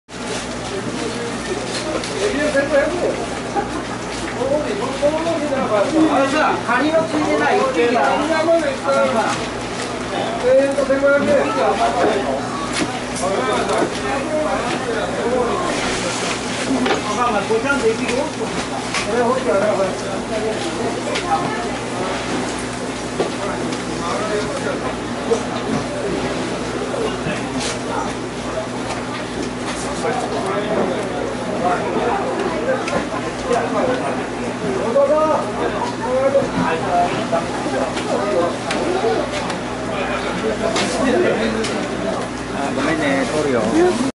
Yanagibashi Wholesale Market - slicing tuna with a sword-knife
Nagoya, Japan October 2015